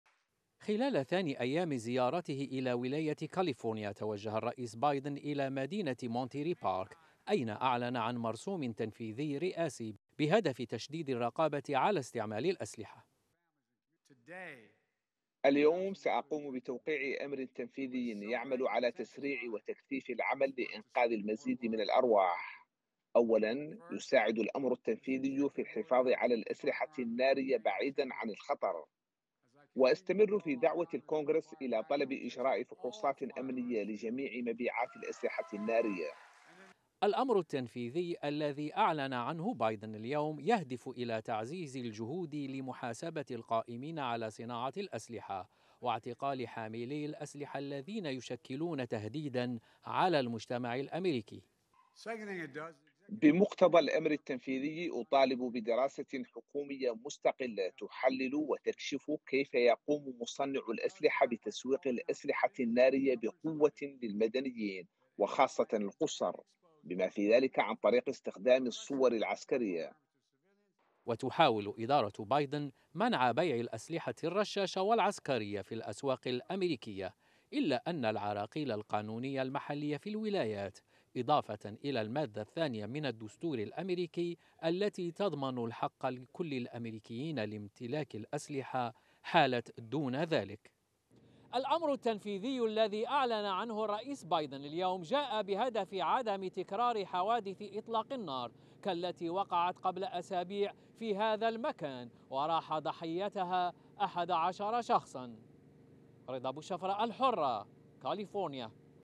بايدن يعلن عن أمر تنفيذي لتشديد الرقابة على الأسلحة
أعلن الرئيس الأميركي، جو بايدن، عن الأمر التنفيذي الرئاسي الذي يهدف إلى تشديد الرقابة على استعمال الأسلحة وتعقب حاملي الأسلحة خصوصا الرشاشة والعسكرية.
وجدد بايدن، خلال كلمة في مونتري بارك بكاليفورنيا حيث التقى ضحايا إطلاق النار، دعوته إلى أعضاء الكونغرس لطلب فحوصات أمنية لجميع مبيعات الأسلحة النارية.

حمل تطبيق "الحرة كونيكت" للتلفزيونات والهواتف الذكية:

أمازون فاير